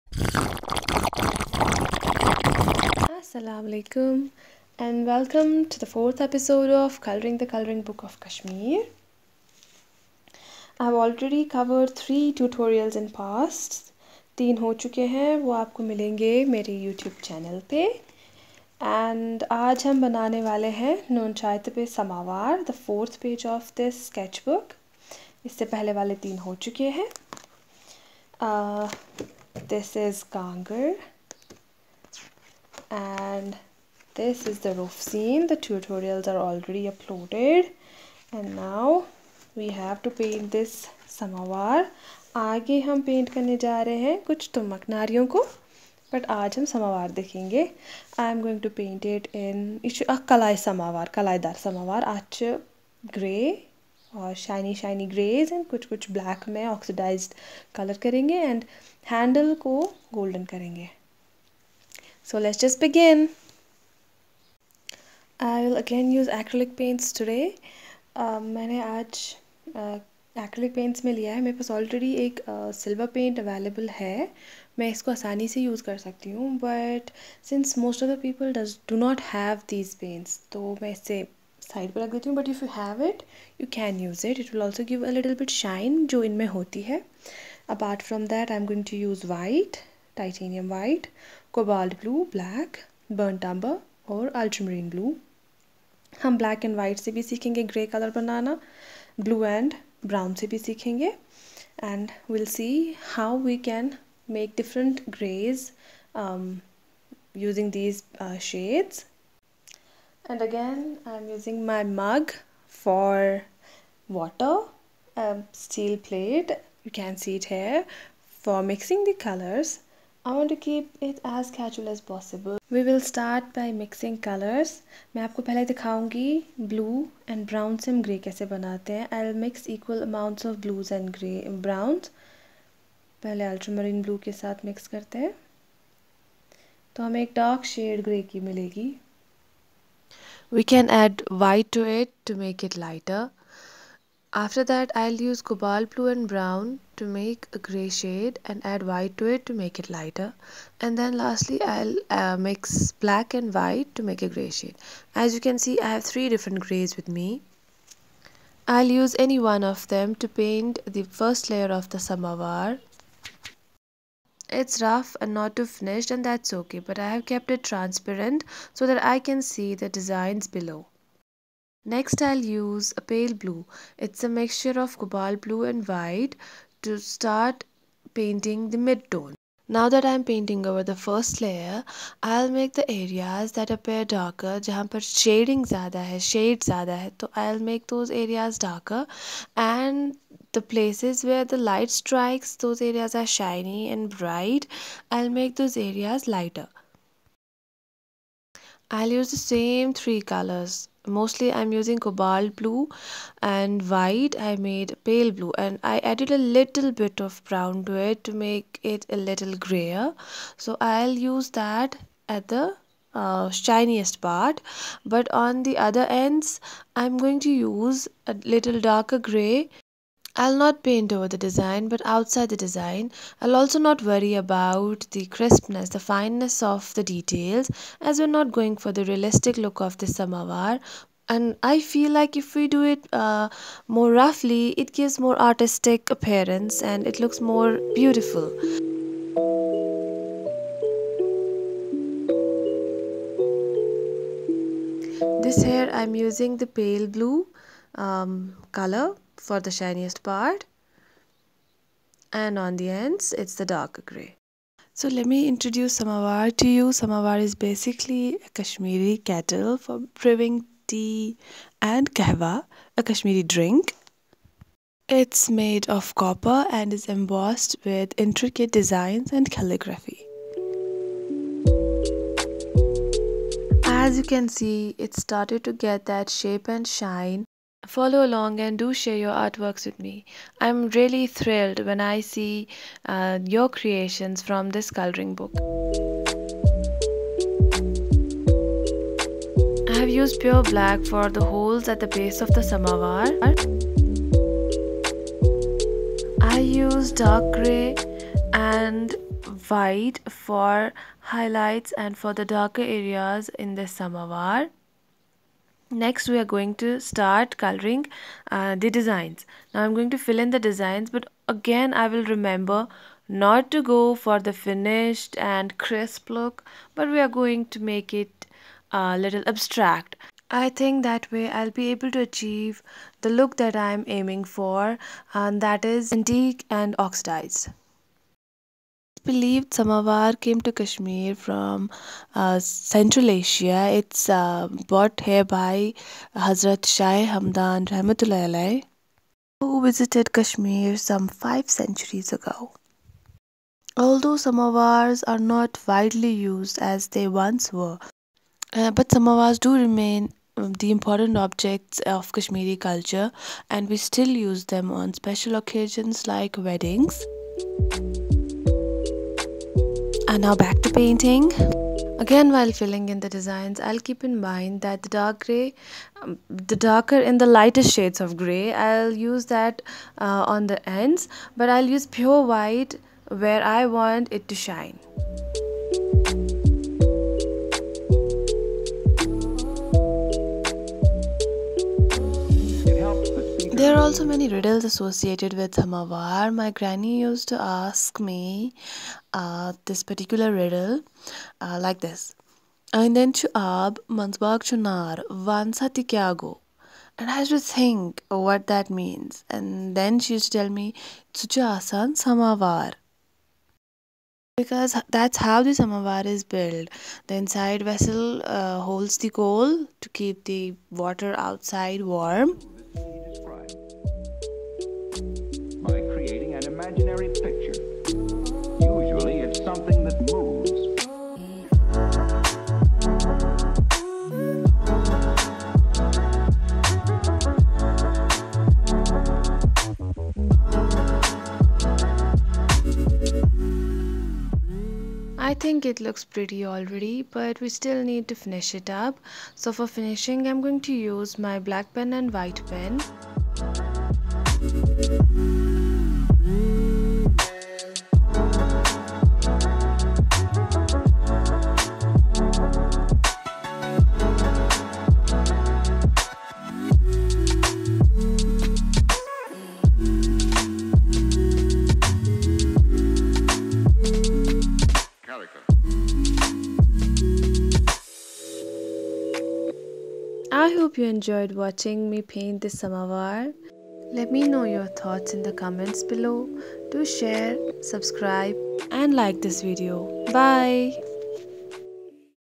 coloring the coloring book of Kashmir/Epi 4/ Painting samovar/Abstract acrylic painting/Impasto art
This is the fourth page from the coloring book.
Materials used:
Acrylic paints.
water
Round Brushes of size: 1,2 and 3
Tissue Paper for cleaning the brushes
Mixing tray. I have used a steel plate. I find it easier to clean.

Other Videos from the series are uploaded in playlist: Coloring the coloring book of kashmir.